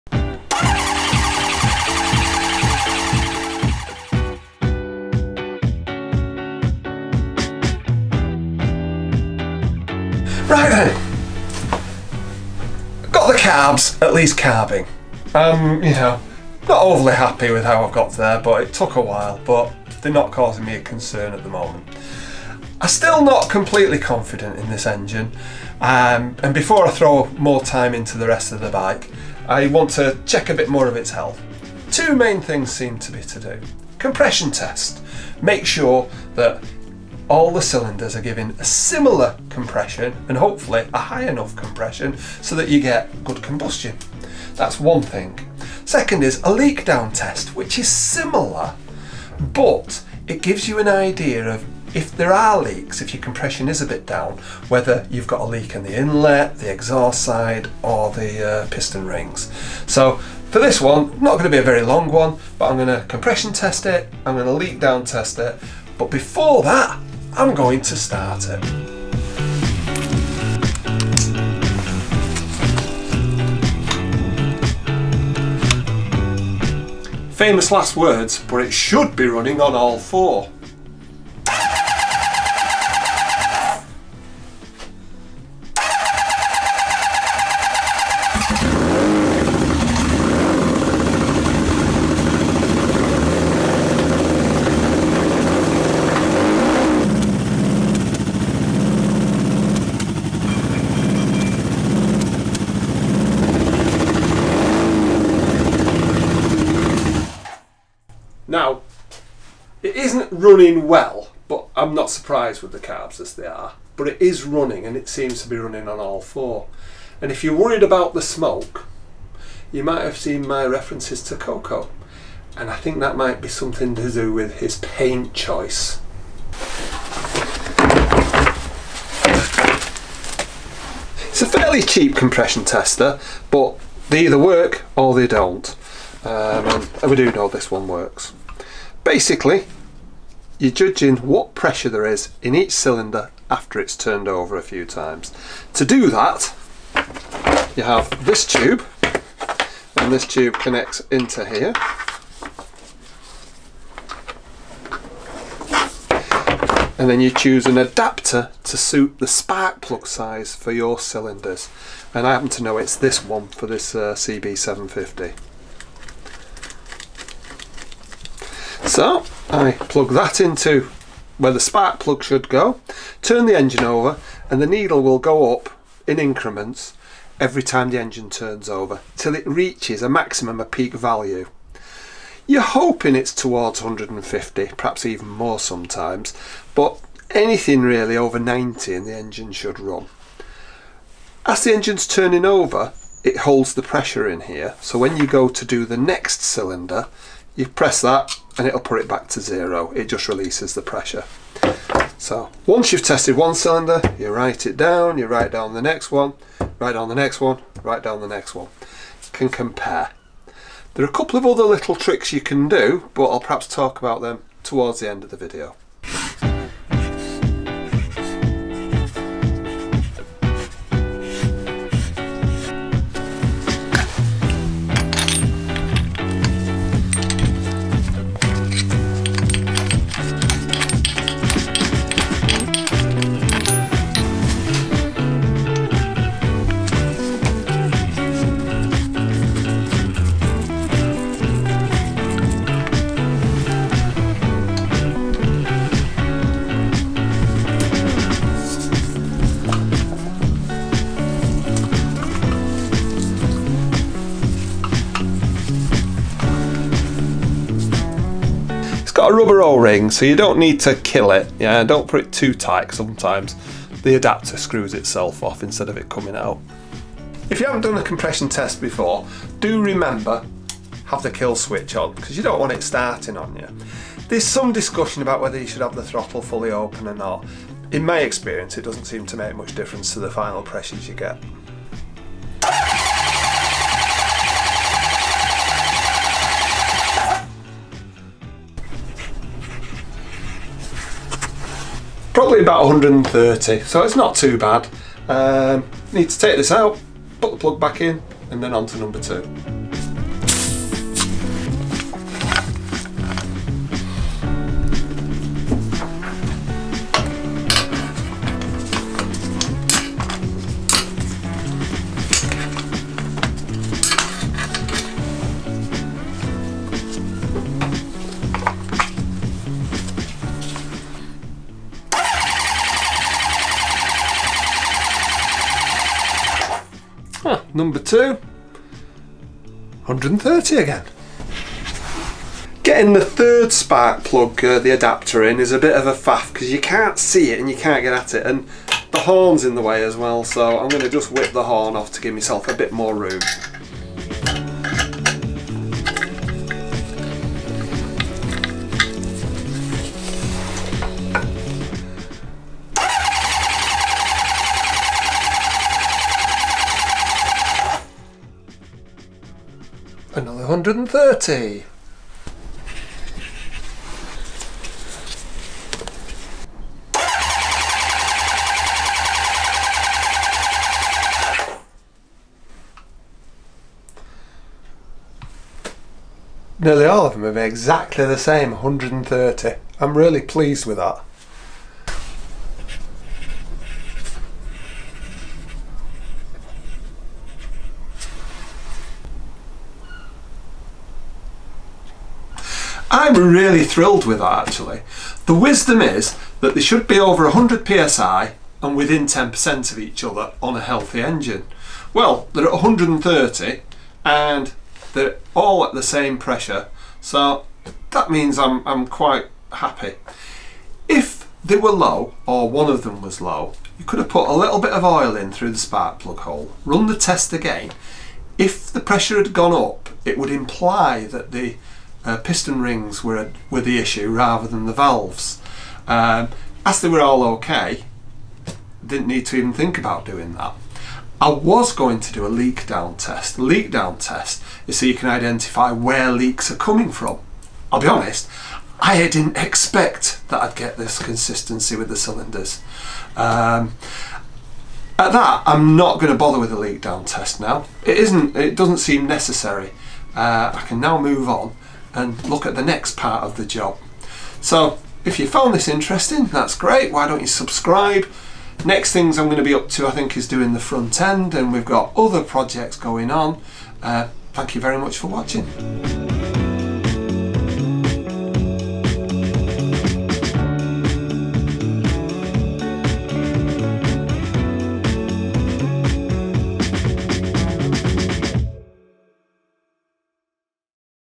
How To Compression Test Motorcycle Engine | Honda CB750 Resto Project Bike | Part 13
In this episode, I compression test my Honda CB750 project bike.  I have spent some time fixing the carburettors and chasing leaking float bowls.  Now that I have managed to have it running on 4 cylinders it was time to see if is sound.

Here's what I used in this video:
Compression tester
Fire Extinguisher

When an engine isn’t run for a number of years it is always likely that either there will be sticky piston rings, or corrosion around one or two of the valves.  This will cause some of the cylinders to be down on compression.

Frequently when you run an engine that hasn’t been run for a long time the sticky rings free up, and the corrosion around some of the valves burns off.  Sometimes it doesn’t!

To do the compression test, one spark plug is removed at a time, so that a line to a pressure gauge can be fitted.  Then the engine is turned over a number of times so that the pressure gauge shows the peak pressure the cylinder is achieving.

The pressure for each cylinder is recorded so that they can be compared against each other.

The wisdom is that the pressure should be above 100psi, the higher the better, but rarely above 140 or so.  There should also be little difference between each cylinder, in that they should be within 10% of each other.

You will have to watch the video to see what result I got!